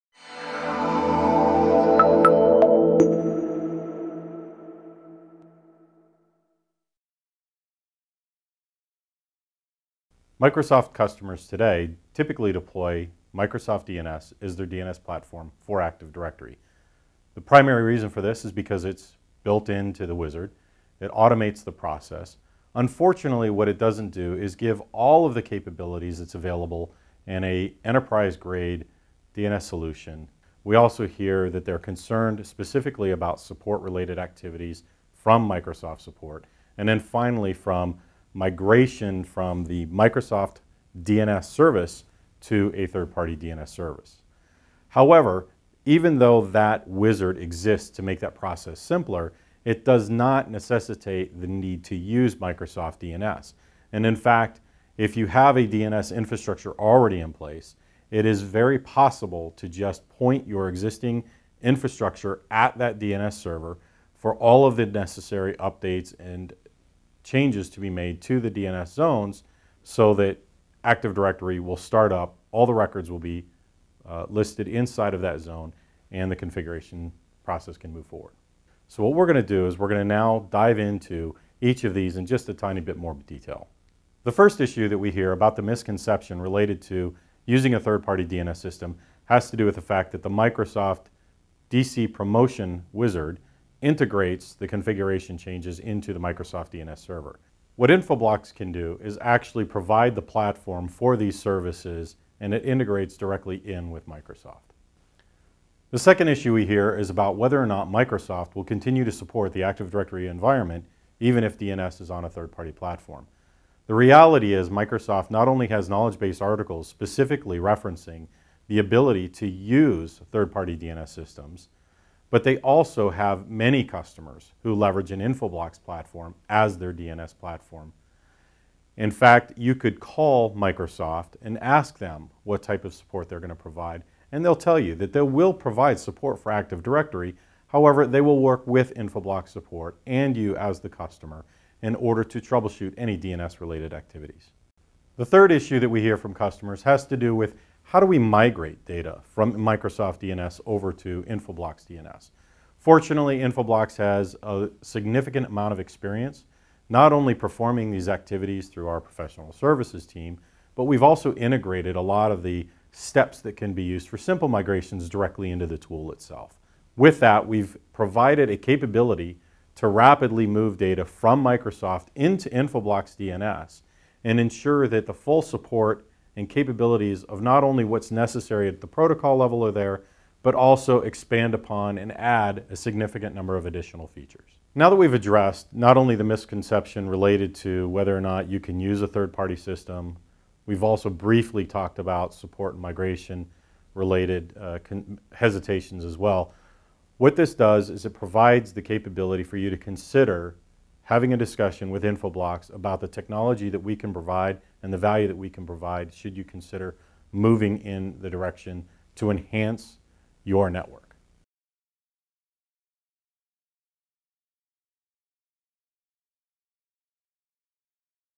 Does Active Directory require Microsoft DNS
In the case of your typical Windows domain network, it is "traditional wisdom" that only Microsoft DNS must be used with Active Directory for internal DNS resolution. Many times this is because:

• It's convenient to use the in-box solution
• The myth that Active Directory requires Microsoft DNS to function properly
• Others are doing Active Directory-integrated DNS, and therefore that way must be the only way to do it

There are some advantages of utilizing Active Directory-integrated DNS for your DNS zone besides the mere convenience of the in-box wizard. Learn more about these advantages here.

Over 7,700 global enterprises and service providers use Infoblox to control their networks.